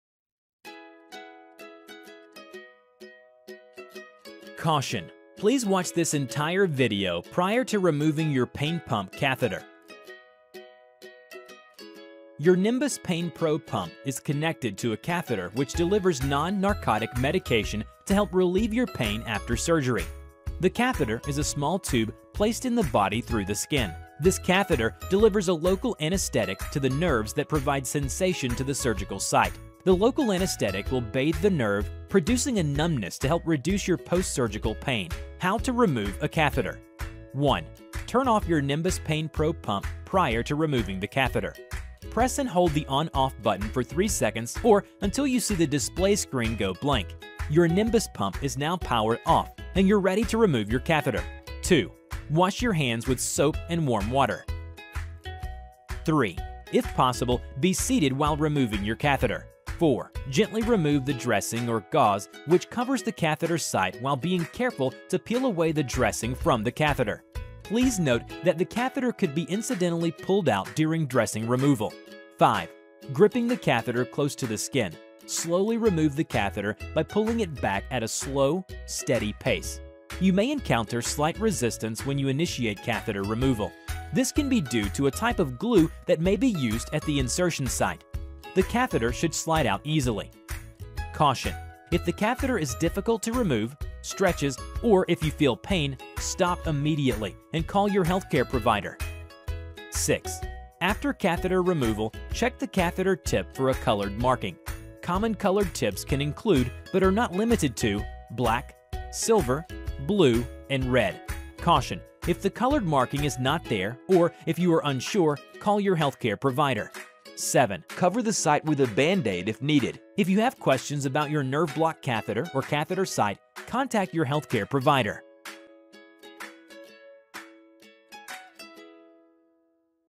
How To Remove Your Nerve Block Catheter
Nimbus PainPRO post op pain pump patient education video on how to remove your peripheral nerve block catheter after your pump infusion is complete. NOTE: Although we want you to return the Nimbus pump and black bag after your infusion is finished, we DO NOT want you to send us the block catheter that you removed. Do you have questions? Call our 24/7 Nimbus PainPRO pump help line at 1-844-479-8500 and press Option 1.